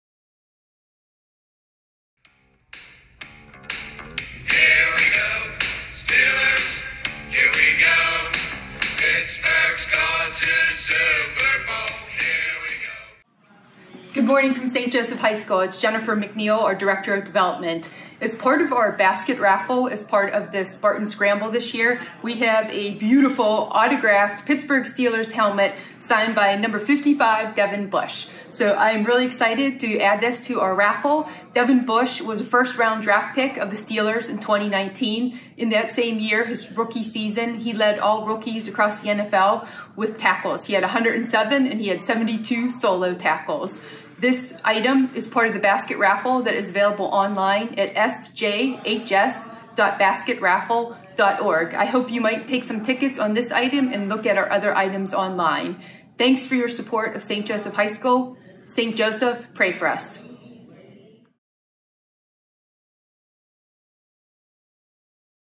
Steeler Devin Bush Helmet in the Spartan Scramble Online Ticket Raffle
Autographed Pittsburgh Steeler Helmet by Devin Bush will be in the 2021 Spartan Scramble Online Basket Raffle.
Devin Bush was a number one Steelers draft pick in 2019. He wears number 55. In 2019, led the team and all NFL rookies in tackles (109) and solo tackles (72).   He is the only Steelers rookie ever to eclipse 100 tackles.